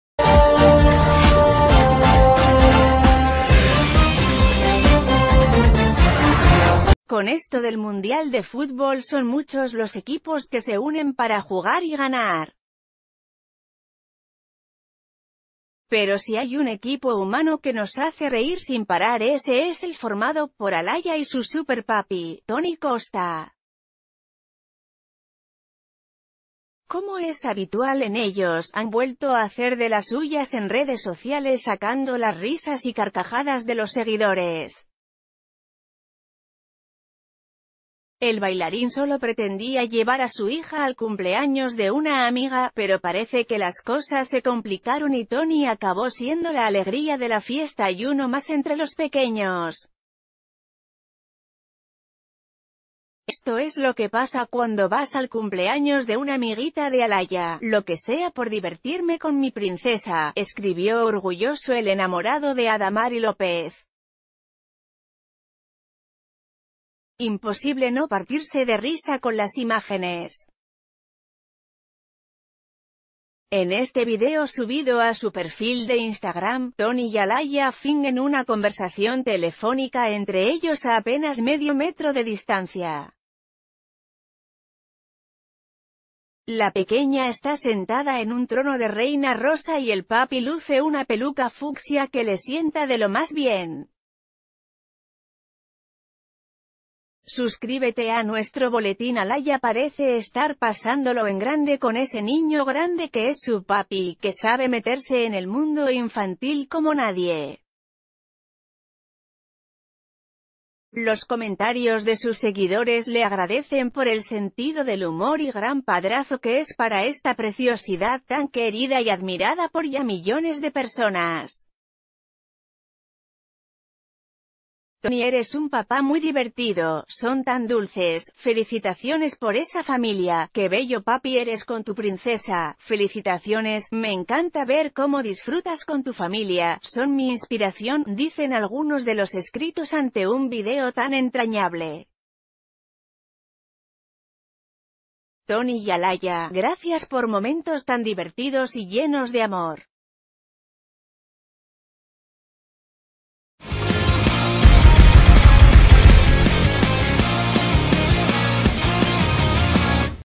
La divertida travesura de Alaïa a su papi, Toni Costa
Con esto del mundial de fútbol son muchos los equipos que se unen para jugar y ganar. Pero si hay un equipo humano que nos hace reír sin parar ése es el formado por Alaïa y su súper papi, Toni Costa.